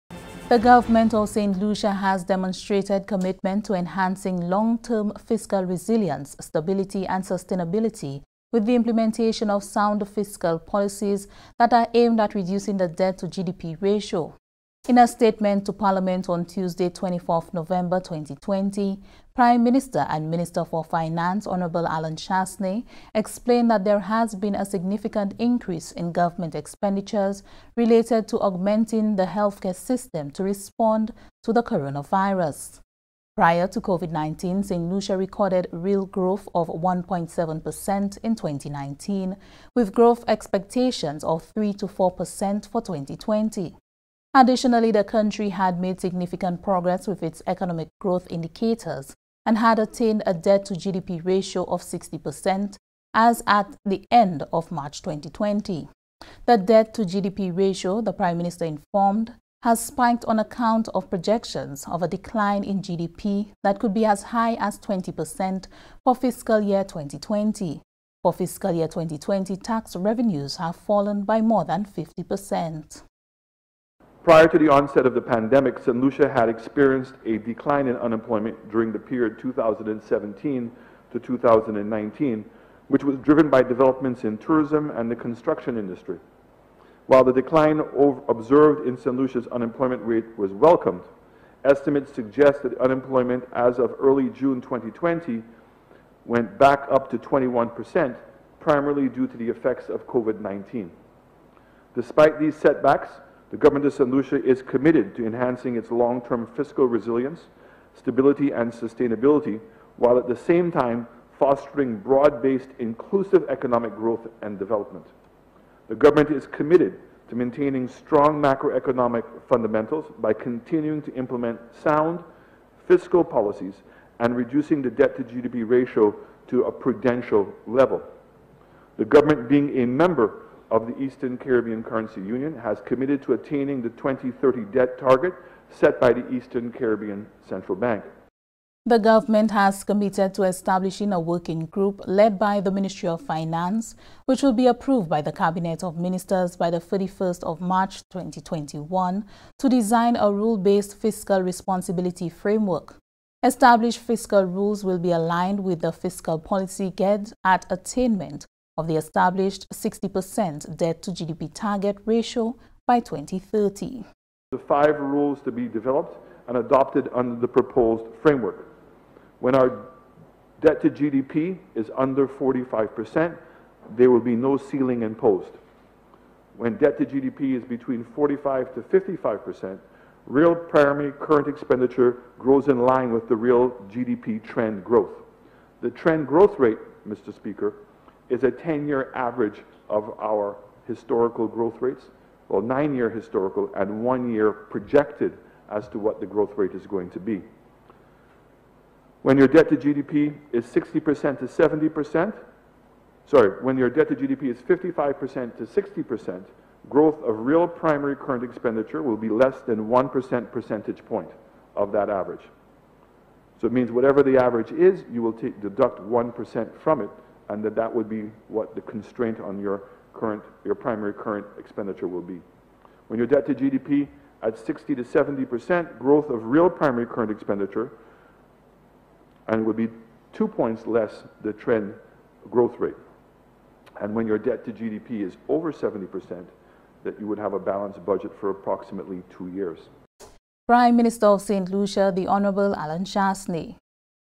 Government implements policies to reduce debt-to-GDP ratio
The Government of Saint Lucia has demonstrated commitment to enhancing long-term fiscal resilience, stability and sustainability with the implementation of sound fiscal policies that are aimed at reducing the debt-to-GDP ratio. In a statement to Parliament on Nov. 24, Prime Minister and Minister for Finance, Hon Allen Chastanet explained that there has been a significant increase in Government expenditures related to augmenting the healthcare system to respond to the coronavirus.